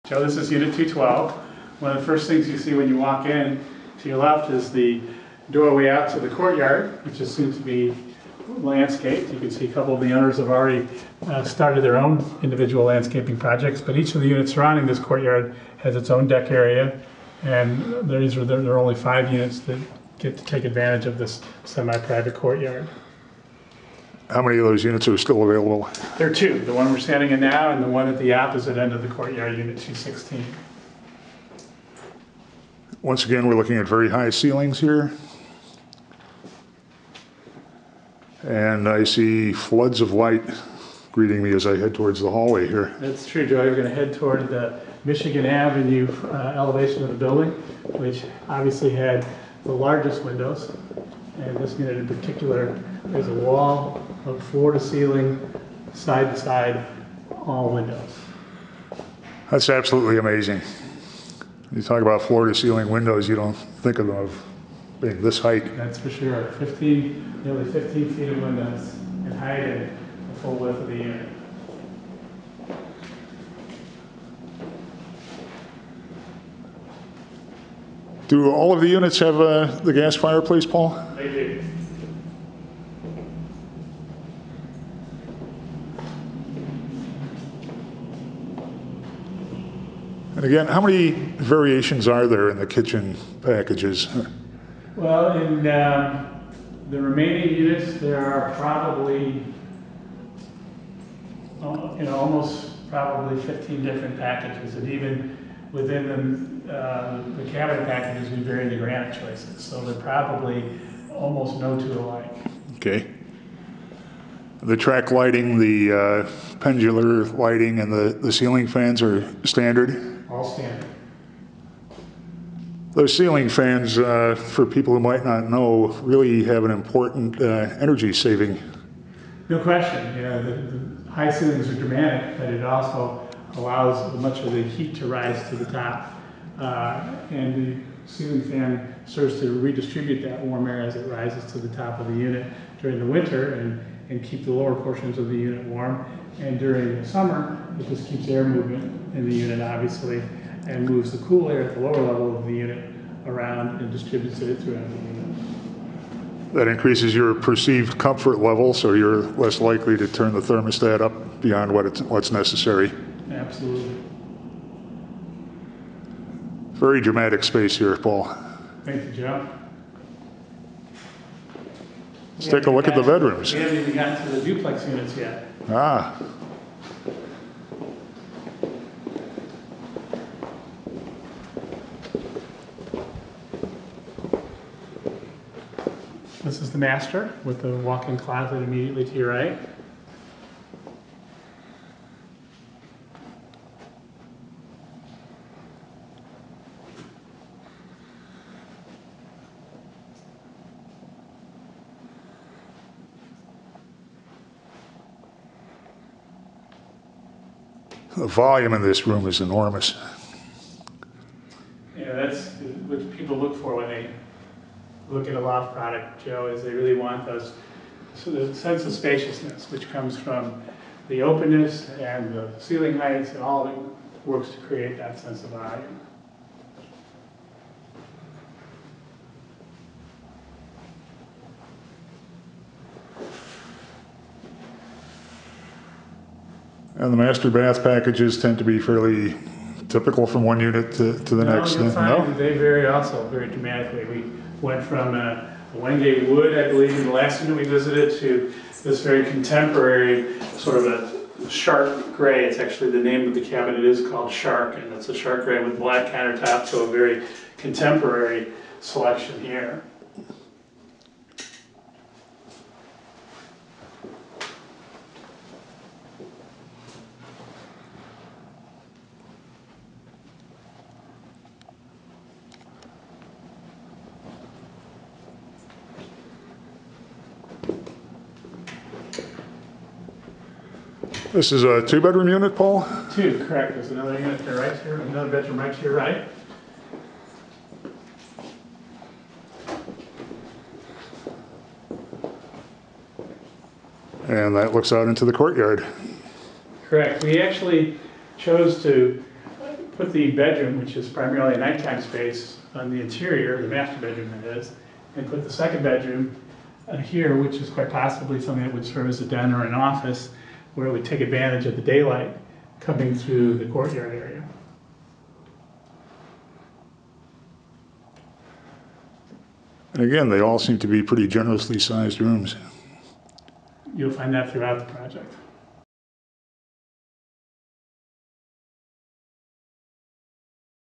A 2 bedroom, 2 bath at Motor Row Lofts Unit 212
Twenty units at this historic loft conversion are slated to be auctioned on November 15th by Sheldon Good and Company.

Developer Paul Zucker took me on an extended tour of the building and some of the units that will be auctioned.

In this video we visit a loft that adds a new dimension to the phrase "floor-to-ceiling windows."